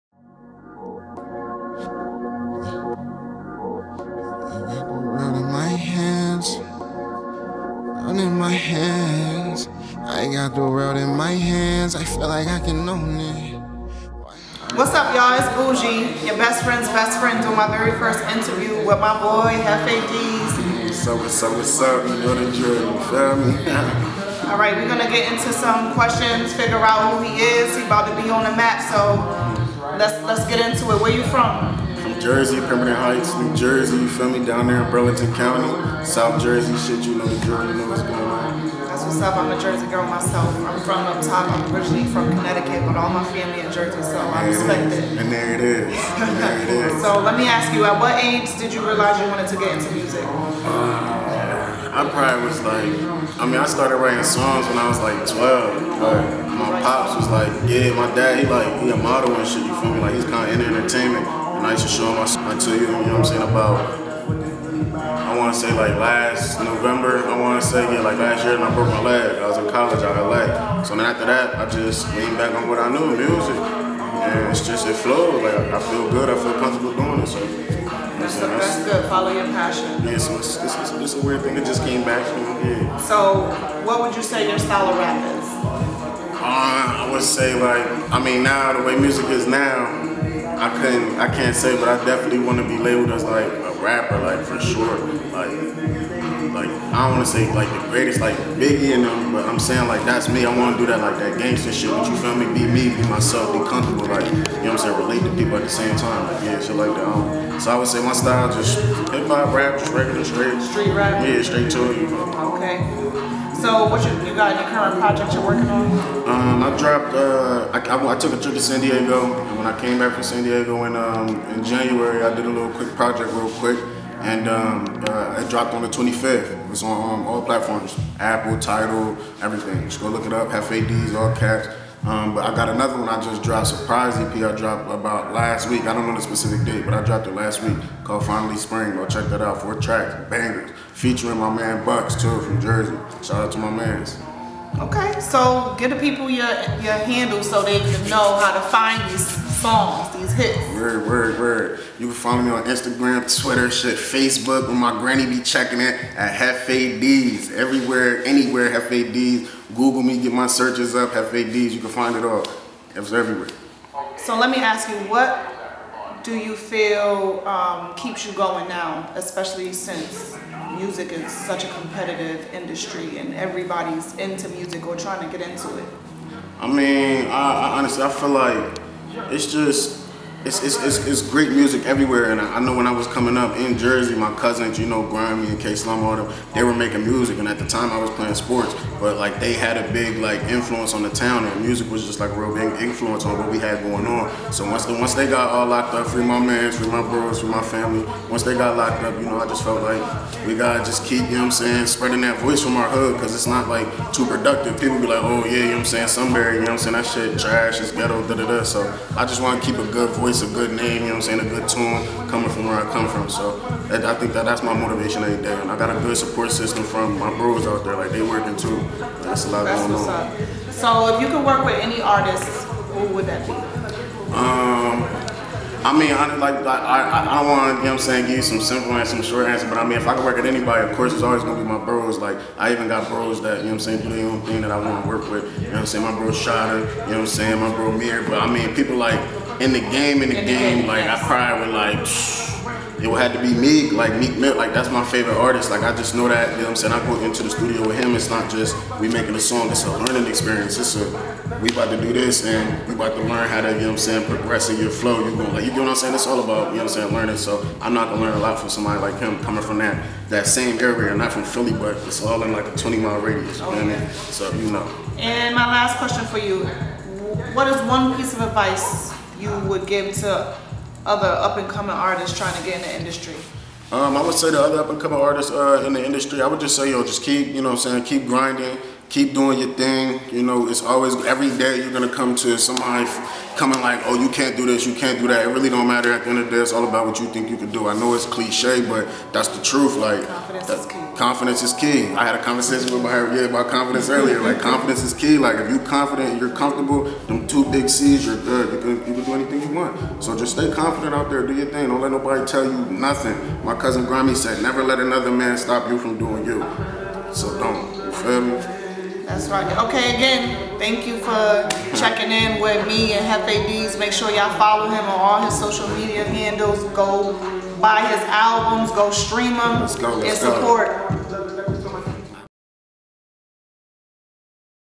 JEFE DEEZ EXCLUSIVE INTERVIEW WITH BOUJI
JEFE DEEZ stops in ATL for an interview with YA GIRL BOUJI